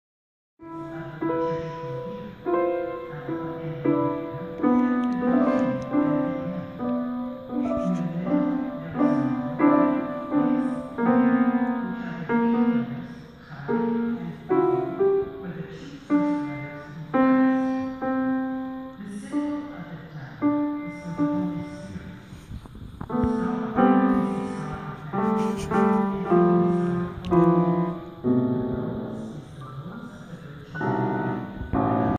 Neiklai the Little Pianist in the making 👏👏
Inside Stensele kyrka Little Man had a go at playing the first biggest piano he has seen yet.